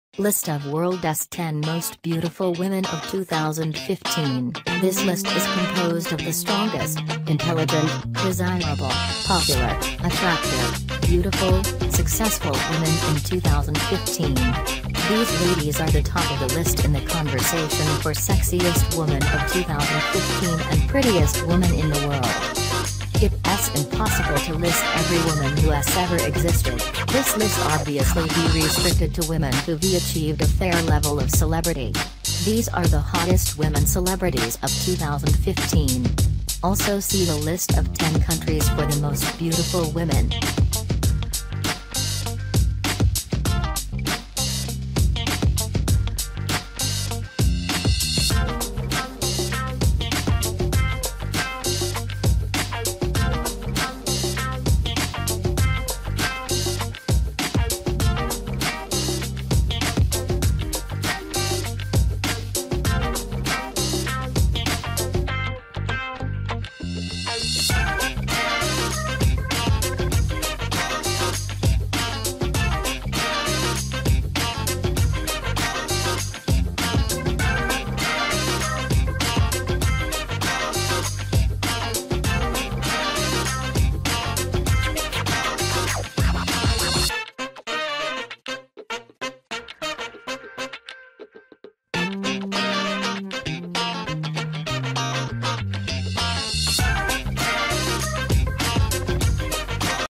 Top 10 World’s Most Beautiful Women of 2015 - 2017
List of world’s 10 most beautiful women of 2015: This list is composed of – The strongest, intelligent, desirable, popular, attractive, beautiful, successful women in 2015. These ladies are the top of the list in the conversation for sexiest woman of 2015 and prettiest woman in the world.

It’s impossible to list every woman who’s ever existed, this list obviously be restricted to women who’ve achieved a fair level of celebrity. These are the hottest women celebrities of 2015. Also see the list of 10 countries for the most beautiful women.

10. Shakira
9. Priyanka Chopra
8. Kate Upton
7. Taylor Swift
6. Gal Gadot
5. Emma Watson
4. Candice Swanepoel
3. Deepika Padukone
2. Jennifer Lopez
1. Shailene Woodley

Wacky Alan